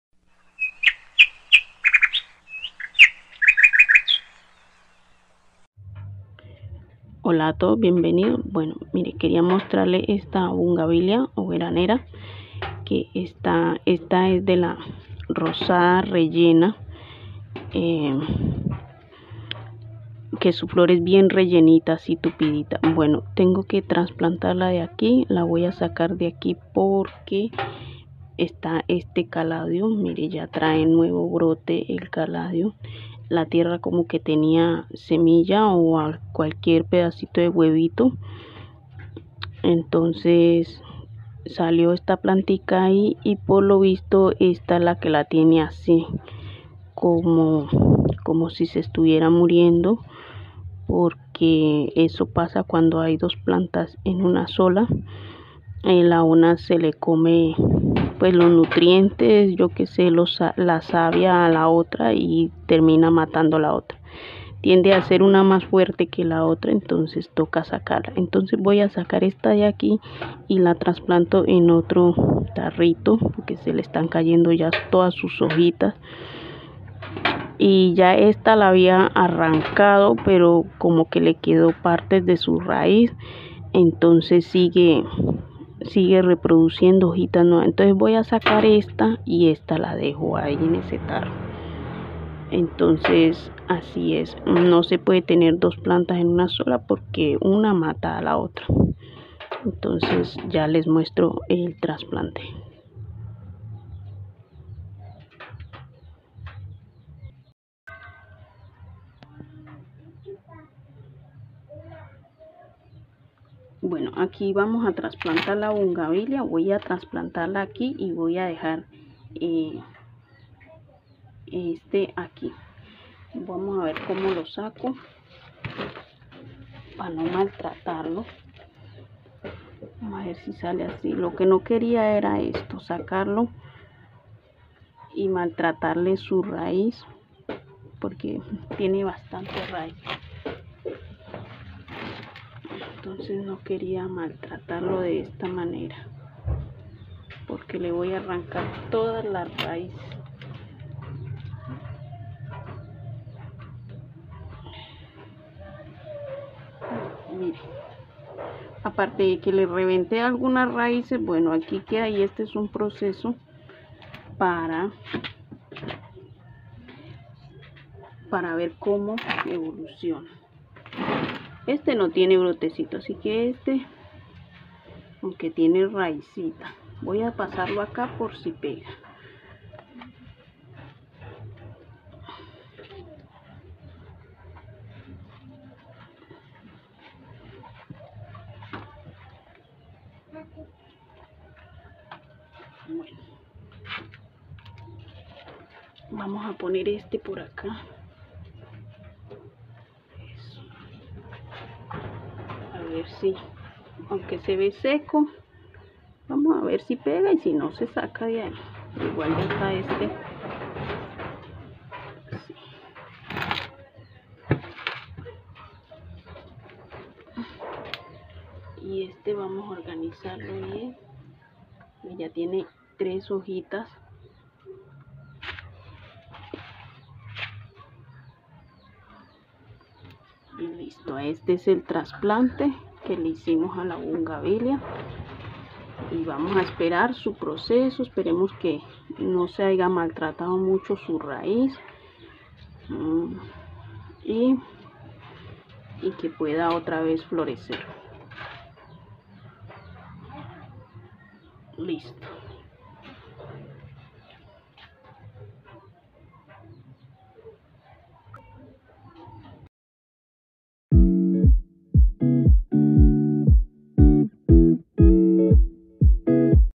Trasplantando y recuperando la bugambilia|🍒Lita🍒
Trasplantando y organizando mi bugambilia
por estar aquí y poder compartir de mi espacio  lo que me gusta y se desde mi esperiencia en mi pequeño jardín

🍒Lita🍒